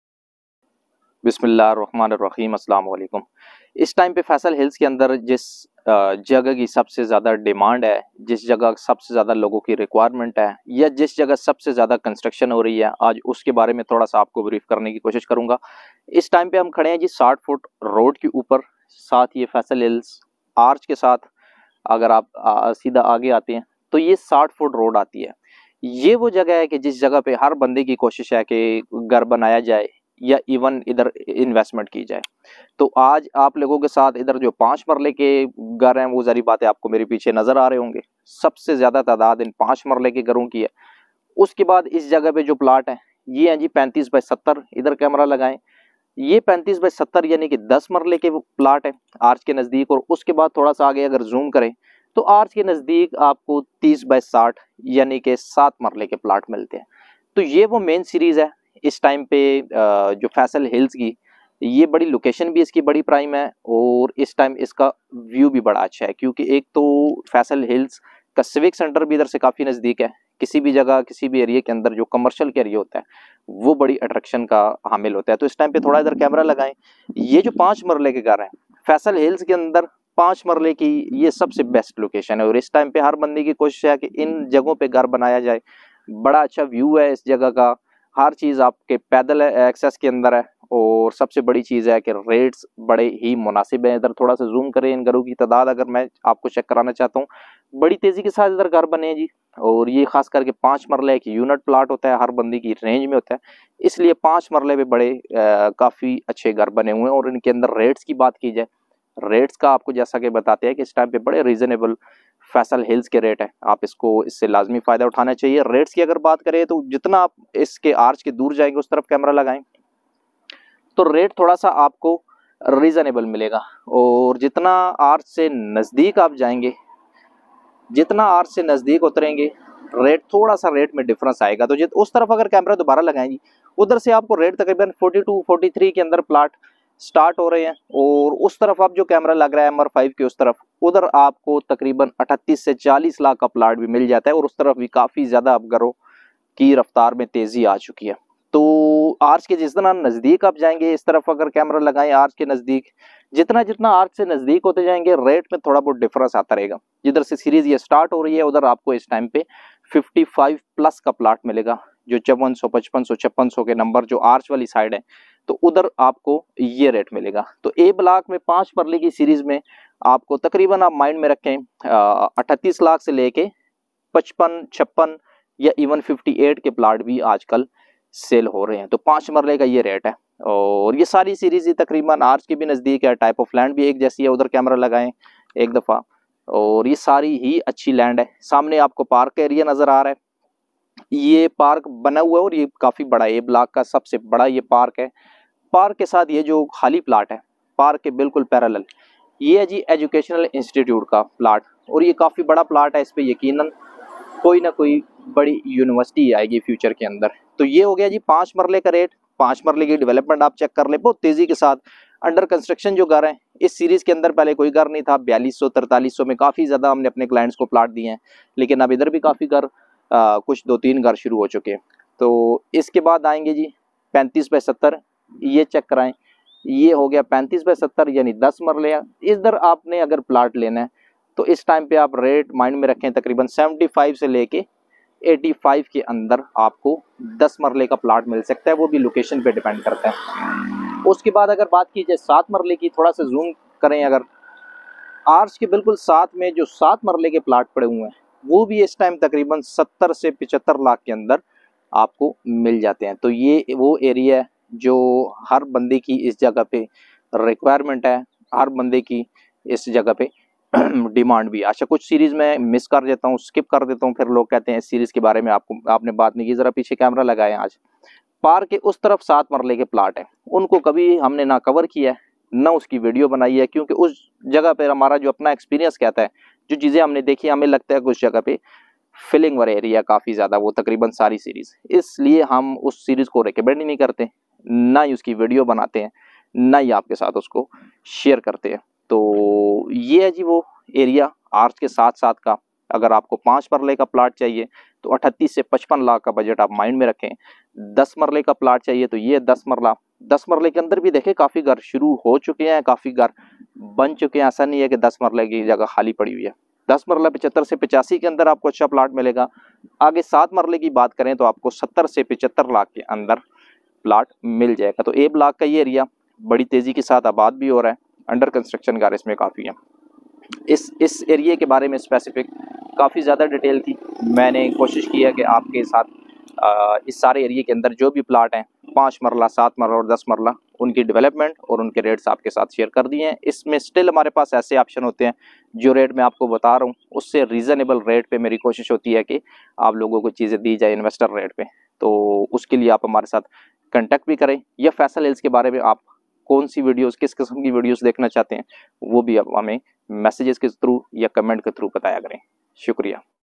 Faisal Hills A Block | Site Visit | Latest Update | Islamabad | plots for sale | Taxila
In this video we will provide you Faisal Hills A block Latest update.we have latest site visit of Faisal Hills Islamabad.Faisal Hills Taxila Block A is ideally located near main gate and Faisal Hills Arch.